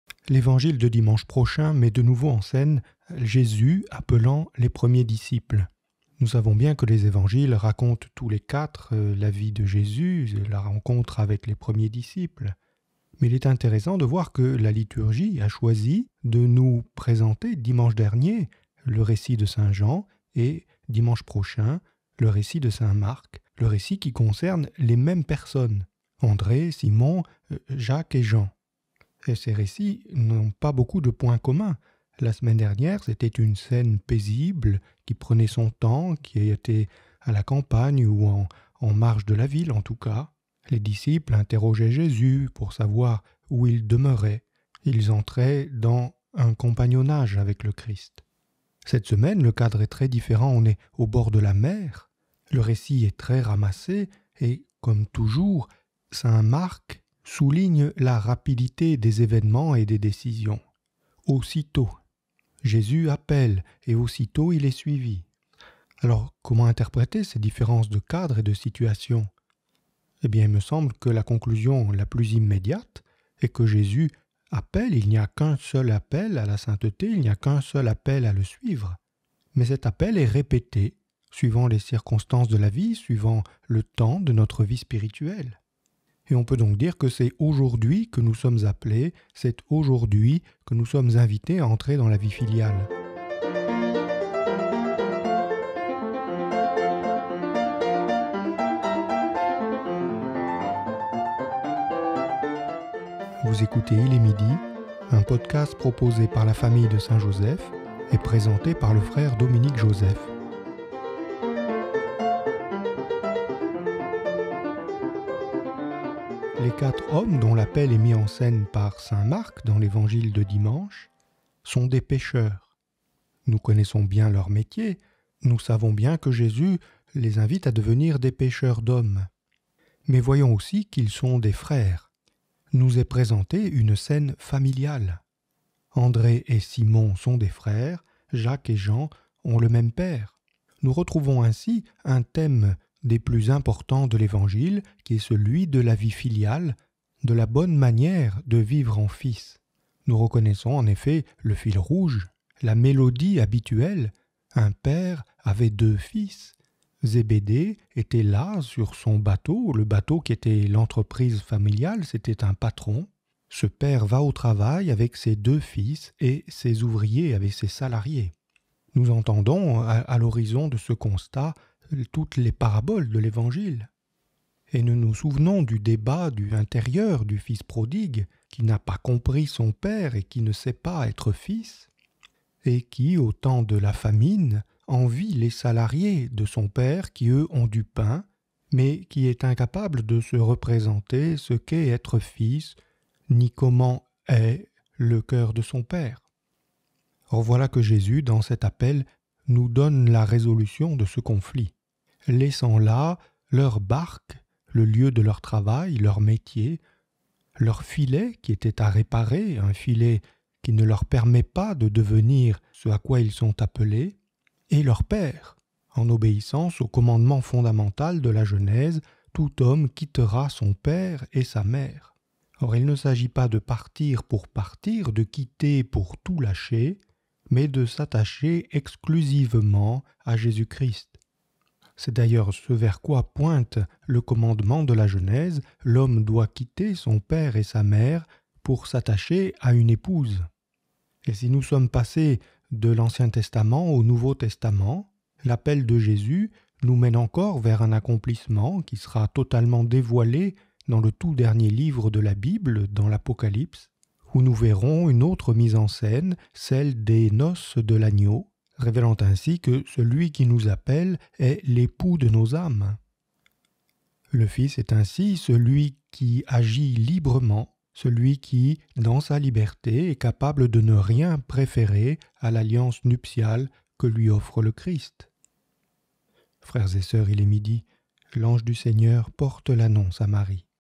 Liberté des fils
Saison 2 Épisode 088 - Liberté des fils
« Un homme avait deux fils » (Mt 21,28)

Répondre à l’appel du Christ oblige à tout quitter, à laisser là le bateau et les filets, la vie coutumière et le père lui-même. Cette scène fondamentale n’est pas le simple éloge de la promptitude des disciples à s’engager, mais la démonstration en actes de la liberté des fils.